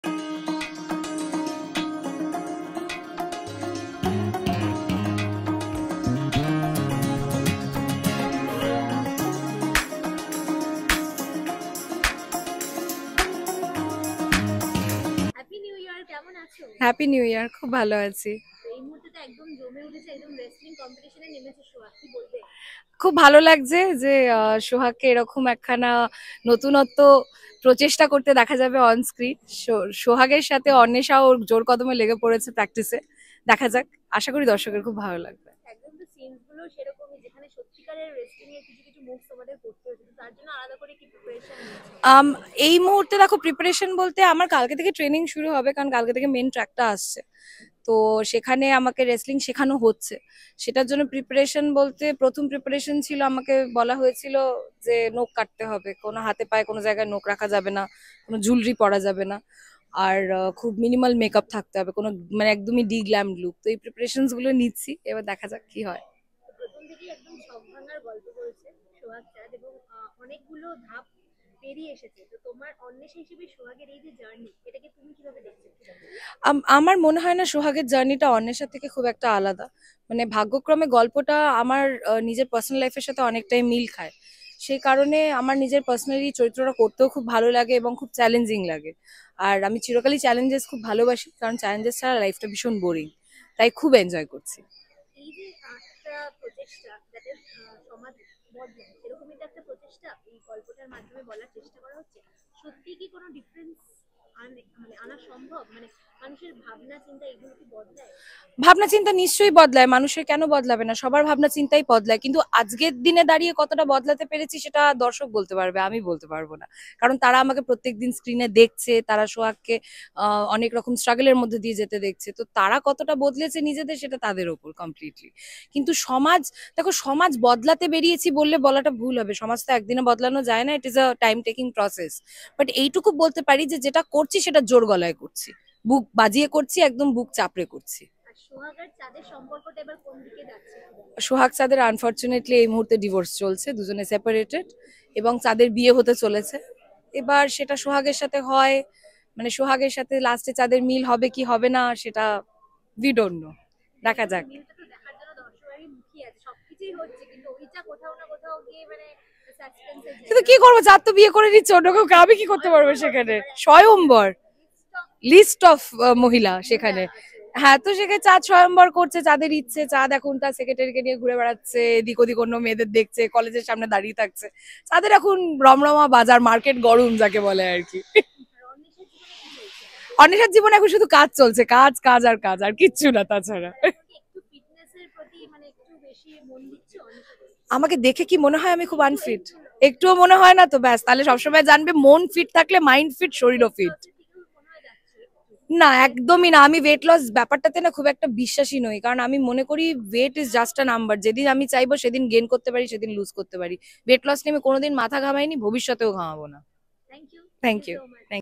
সোহাগ আর অন্বেষার লড়াই অনেকটাই এক: অকপট অন্বেষা| নতুন চমক নিয়ে আড্ডায় সোহাগ| Sohag Chaad
সোহাগ আর অন্বেষার লড়াইয়ের মধ্যে অনেকটাই মিল আছে| স্বীকার করলেন অভিনেত্রী| বাস্তবে নিজের ওজন নিয়ে খুব খুশি অভিনেত্রী| তিনি যেমন তেমনটাই থাকতে চান বাস্তব জীবনে| এই মুহূর্তে জমে উঠেছে সোহাগ চাঁদ| কুস্তির ময়দানে নেমেছে সোহাগ| আগামী দিনের চমক নিয়ে আড্ডায় অন্বেষা

Videography & Edit: Shreyasree Das

An exclusive interview with actor Anwesha Chakraborty where she talks about the upcoming twists in the serial Sohag Chaad